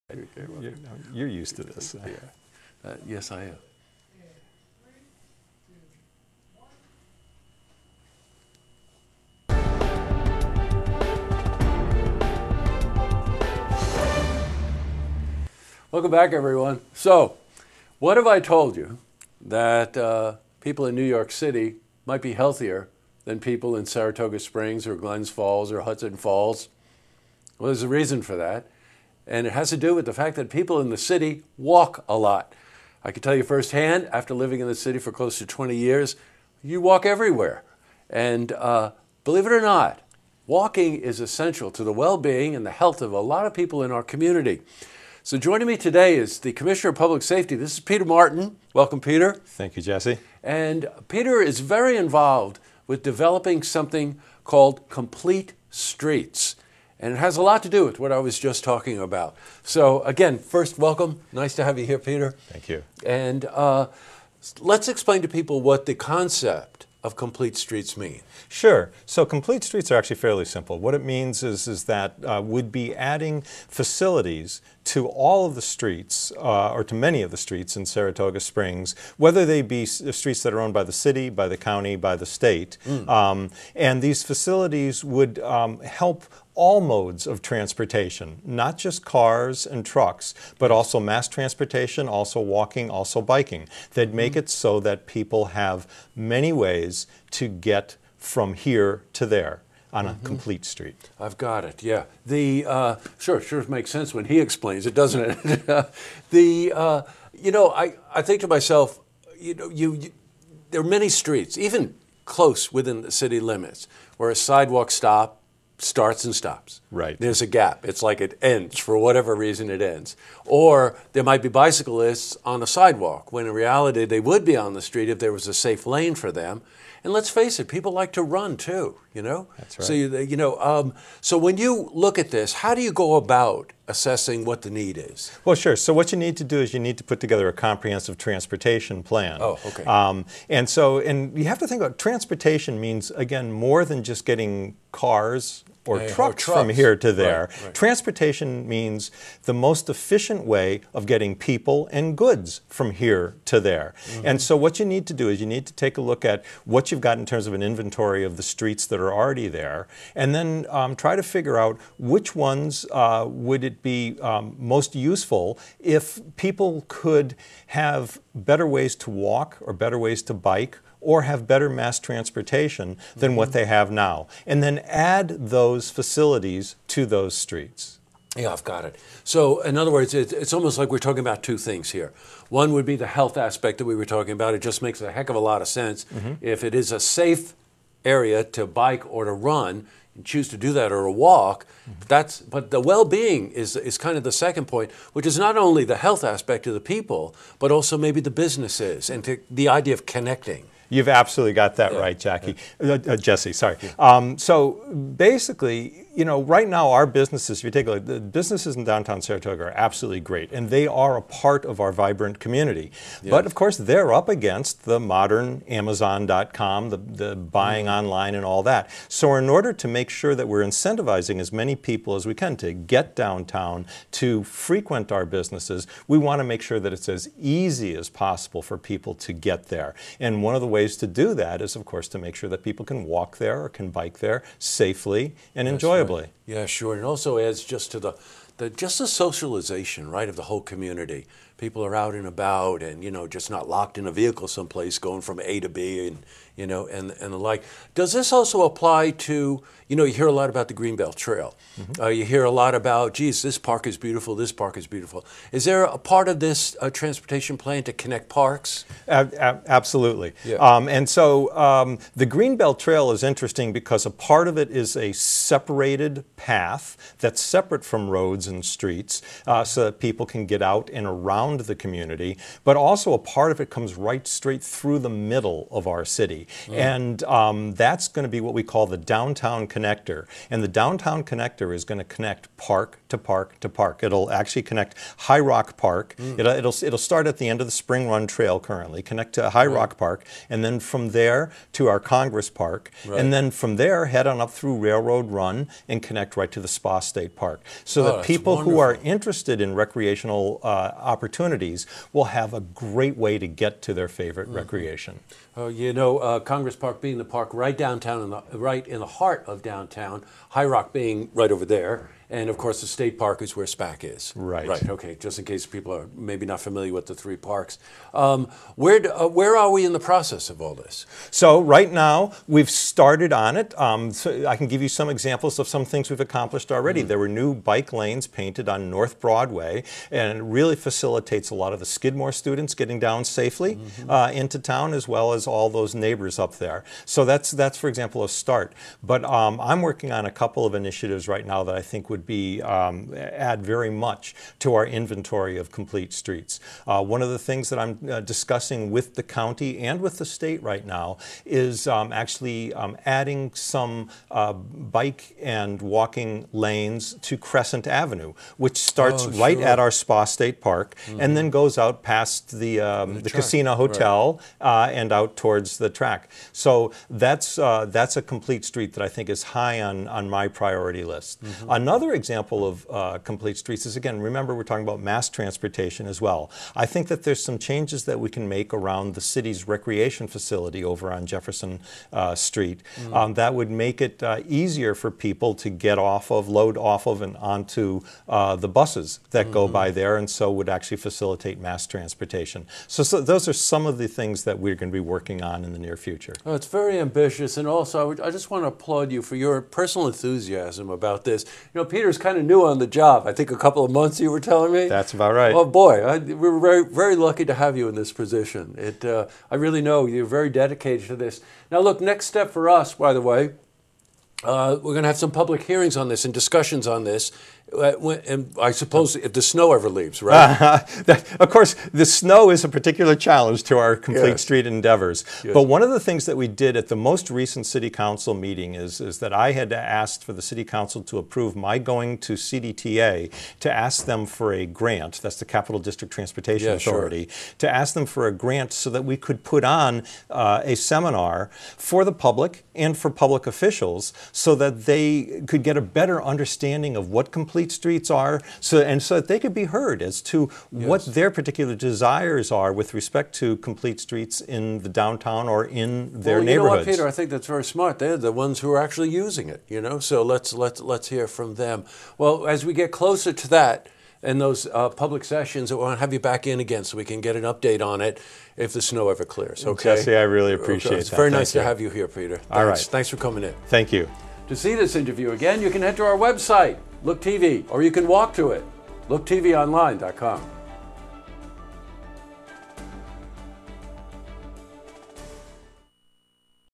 Peter Martin on Complete Streets in Saratoga--Interview 2018
Join Look TV as we sit down with Peter Martin, Commissioner for Public Safety for Saratoga Springs. He's here to give us an update on how the Complete Streets Initiative is Progressing.